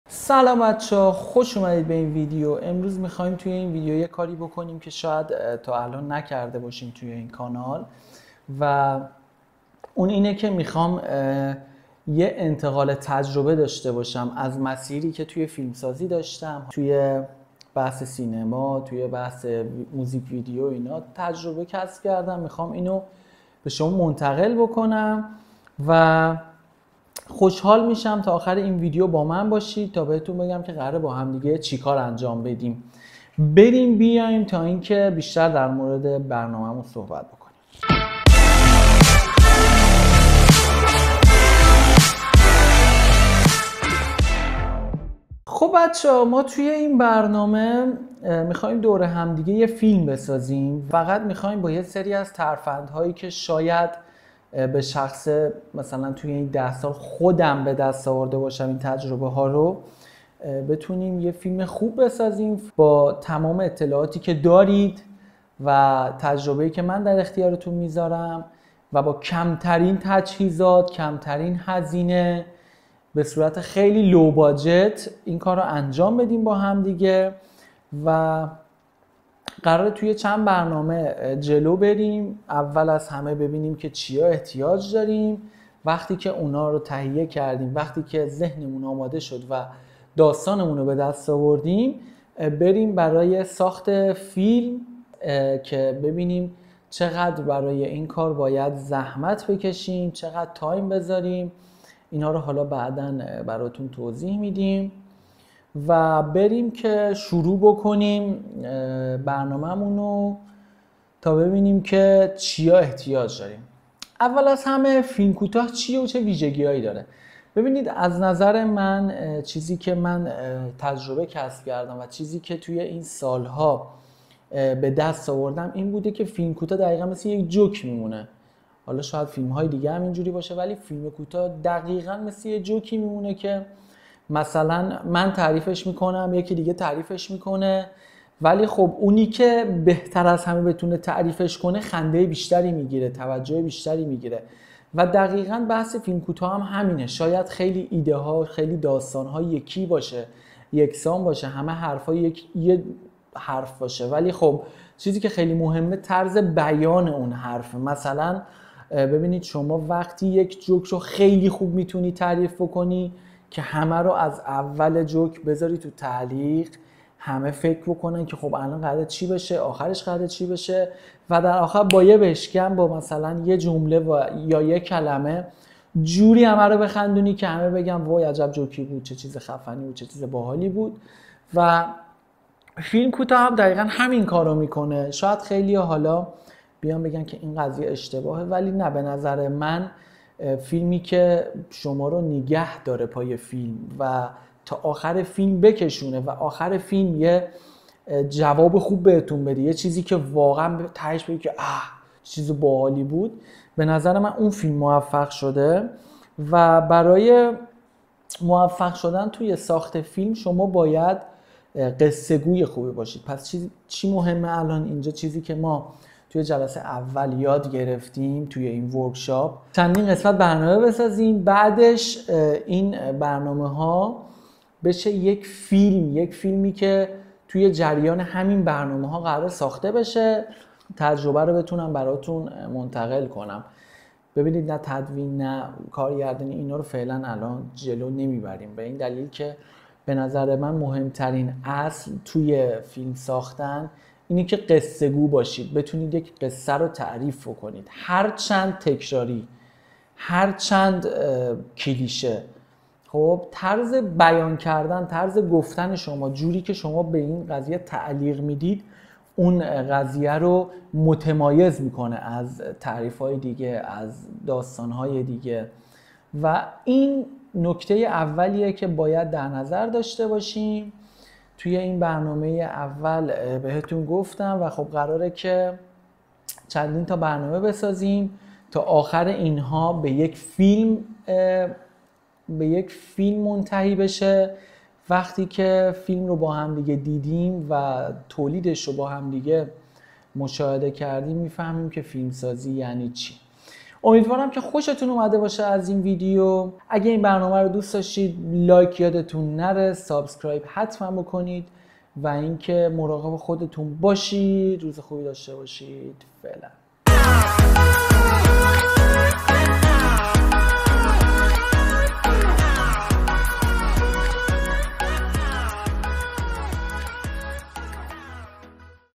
How To Be A Low Budget Filmmaker EP1-Story Teller چگونه فیلمساز شویم؟ قسمت اول - قصه گویی
قسمت اول از برنامه چگونه فیلمساز شویم و فیلم بسازیم؟
موضوع این قسمت : قصه گویی
لایک و کامنت فراموش نشه
همین الآن سابسکرایب کنید

How To Be A Filmmaker & Make A Film EP1-Story Teller
Special Video Workshop To Be A Filmmaker For Free!